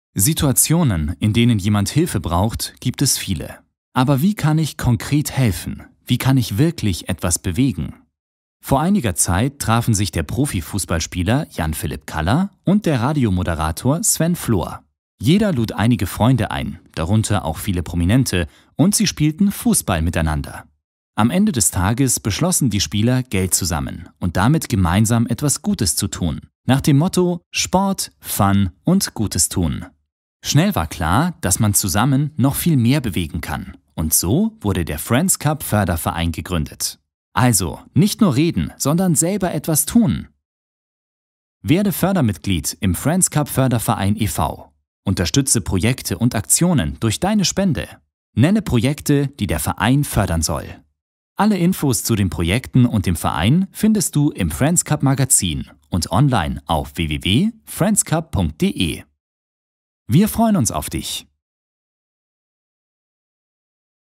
FRIENDS CUP - Der Film
Die Idee:
Jan-Philipp Kalla und Sven Flohr haben im Frühjahr 2016 eine Idee in die Tat umgesetzt. Beide sind aufgrund ihrer öffentlichen Präsenz sehr stark engagiert, hilfsbedürftige Projekte zu unterstützen. Mit der Idee vom FRIENDS CUP haben sie das Angenehme mit dem notwendig Nützlichen verbunden. Einmal im Monat laden sie gemeinsam Freunde und Partner zu einem besonderen Event ein. Das Motto lautet stets: „Gemeinsam Sport treiben und das mit der nötigen Portion Spaß!“

Bei der Auswahl der Sportarten ist alles möglich. So wird zusammen Fußball, Handball oder Volleyball gespielt. Aber auch Disziplinen wie Bagger fahren, Kart fahren, Eishockey oder Minigolf stehen auf dem Programm. Die Gäste erleben 3 Stunden fernab vom Alltagsstress jede Menge Spaß. Jeder Gast darf am Ende der Veranstaltung einen Betrag seiner Wahl ganz anonym in den Klingelbeutel werfen. Die Gesamteinnahmen des Tages werden dann gespendet! Der Spendenzweck wird von den Gästen zusammen beschlossen.
Alle Infos zu den Events gibt es hier: EVENTS

Der Verein:
Schon nach den ersten 3 Events war klar, dass diese Art „Gutes Tun“ viel mehr Potential hat und daher haben Stammgäste der Events zusammen den gemeinnützigen Verein FRIENDS CUP Förderverein e.V. gegründet. Diese Plattform ist der offizielle Träger aller Events und Aktivitäten. Neben Privatpersonen haben hier auch Firmen die Möglichkeit sich zu engagieren.

Wir würden uns freuen, wenn wir auch Sie und Euch begeistern können, uns bei diesem Projekt zu unterstützen. Werden auch Sie als Fördermitglied Teil der FRIENDS CUP Familie!